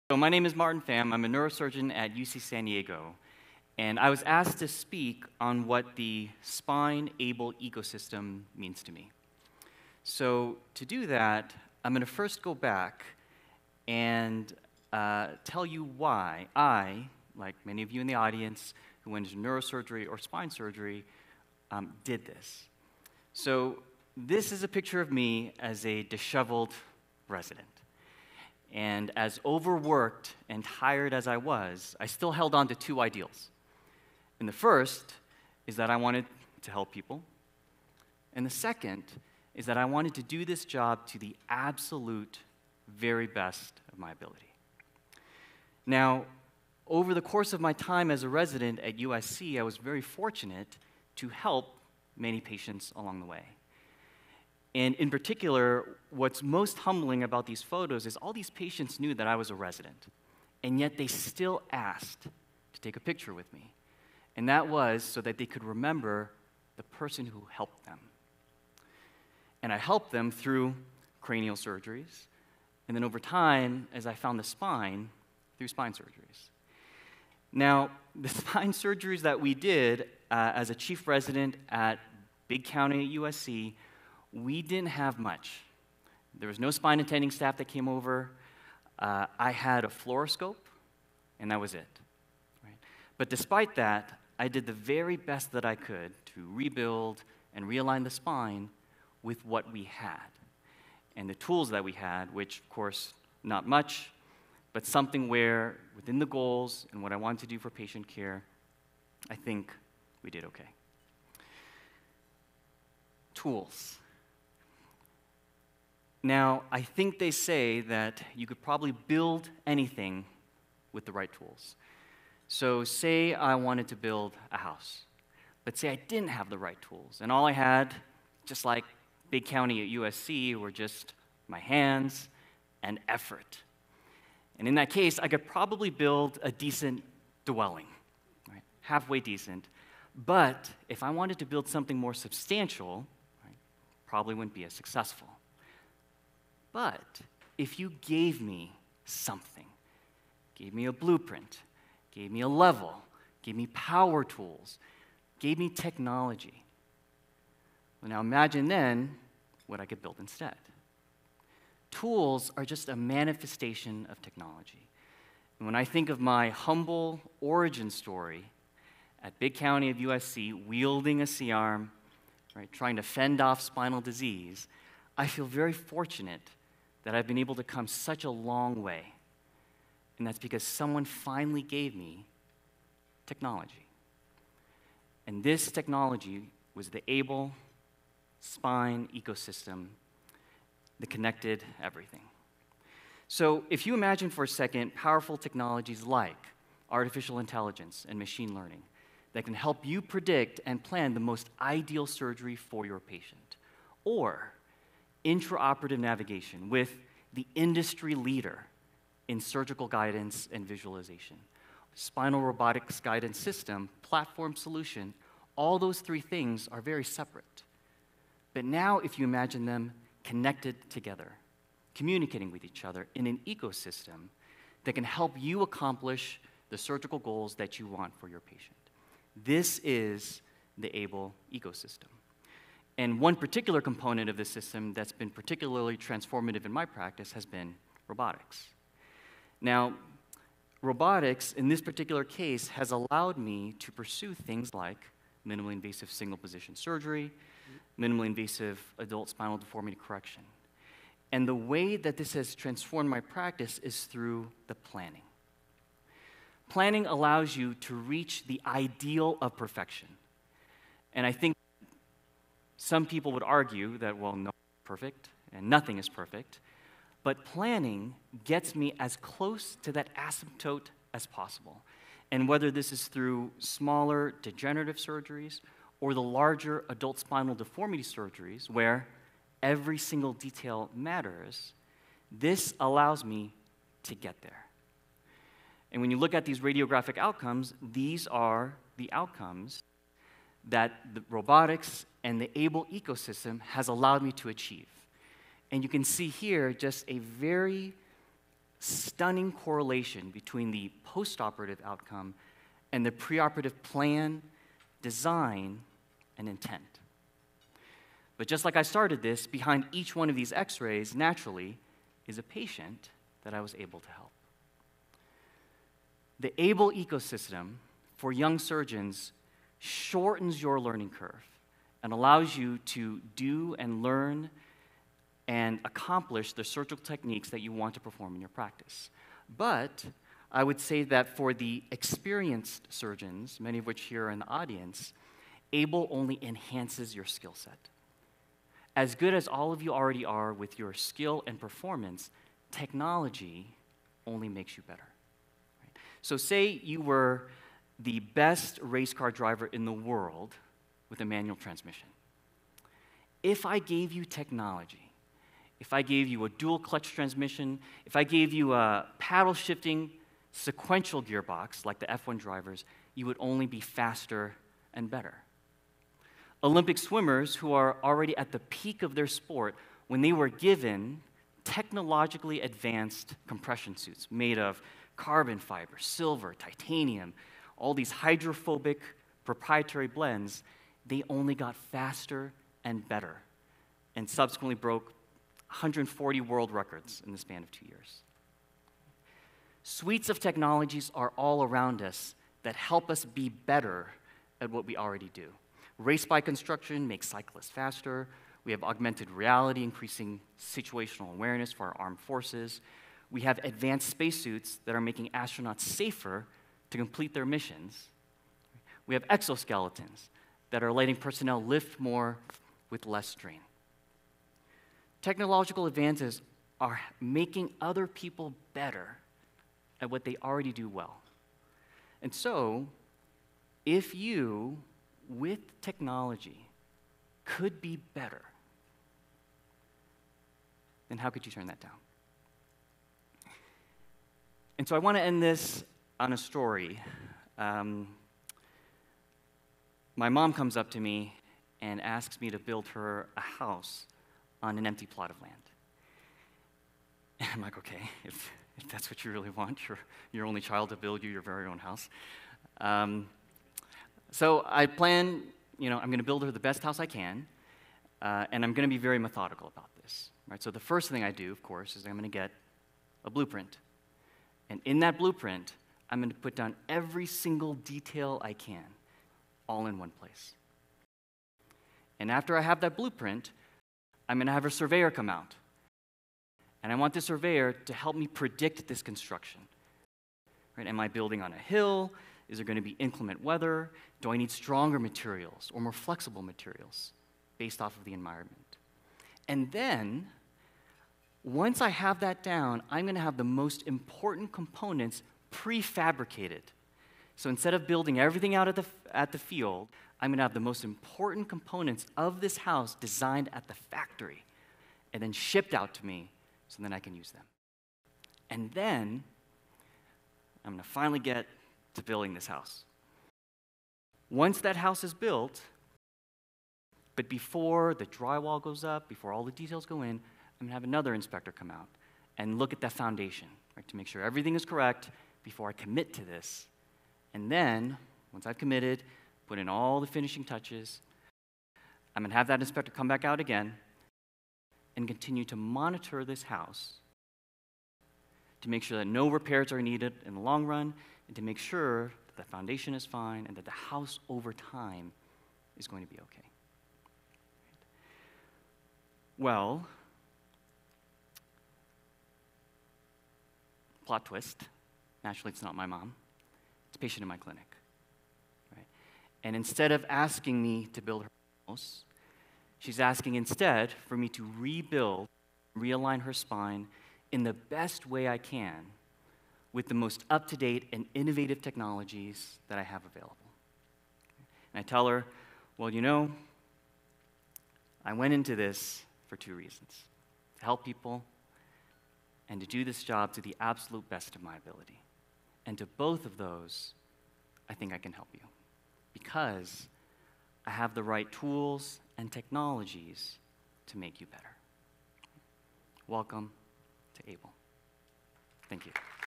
MDT Optimized Surgery with Aible - An Ecosystem of Spine Technologies (Robotics, Navigation, and AI)
Dr. Pham discusses how enabling technologies brought together in a cohesive ecosystem offer advanced integrated solutions to provide the most specific and tailored care to the spine patient. Presented as part of the Plenary Session at the Optimized Surgery with Aible meeting in 2023.

Martin H. Pham, MD is an Assistant Professor in the Department of Neurosurgery at UC San Diego. He is a neurosurgeon who focuses on the neurosurgical evaluation and treatment of spinal disorders.